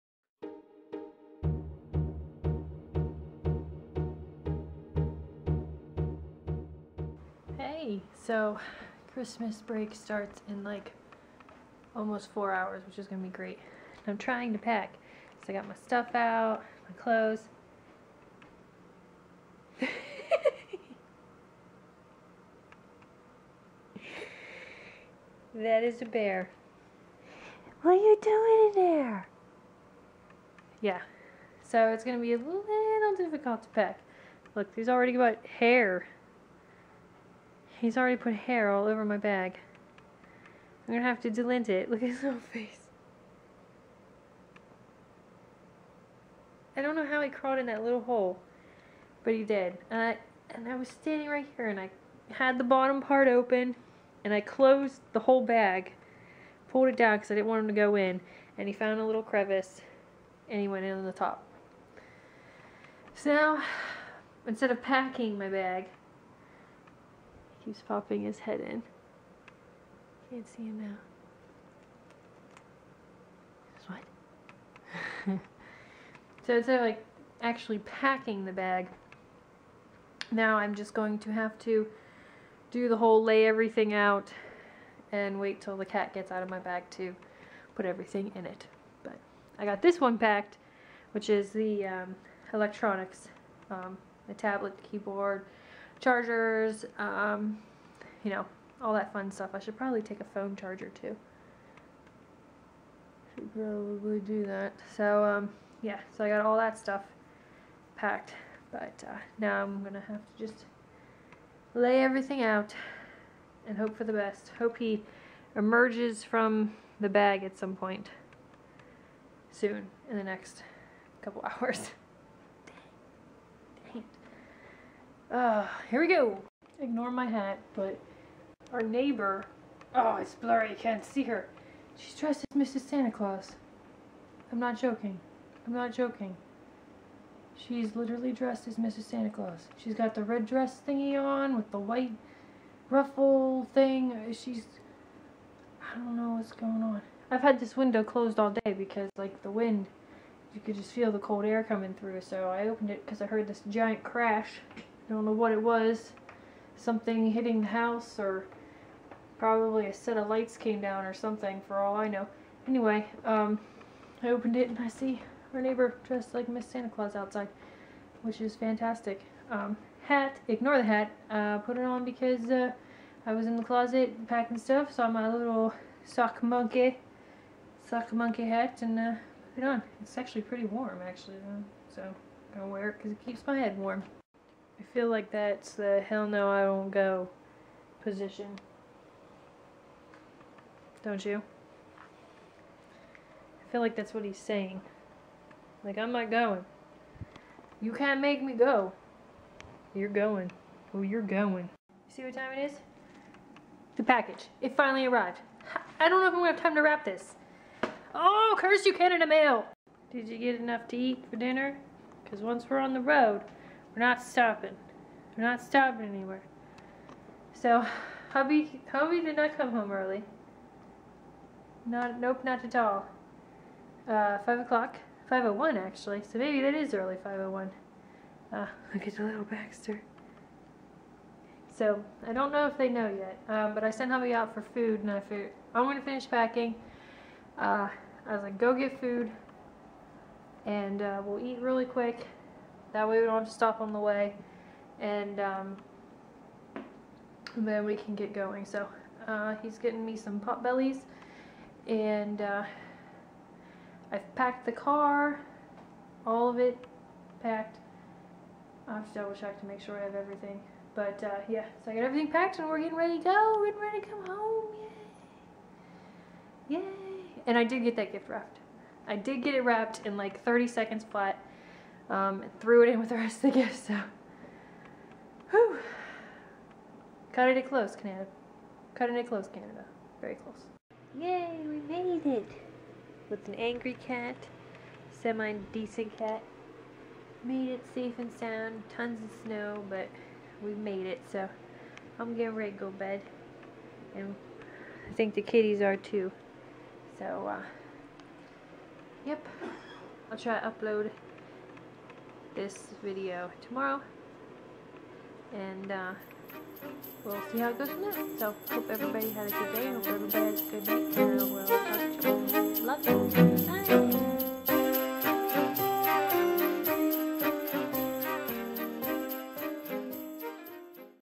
12.21.2012 ~ Sock Monkey Santa
Packing is always fun with cats. Everyone is finally getting in the Christmas spirit, even my neighbors. Yea for SNOW... finally!! Canada Post finally came through, be it the very last minute.  Made it home for Christmas break and SO excited to see all my family!!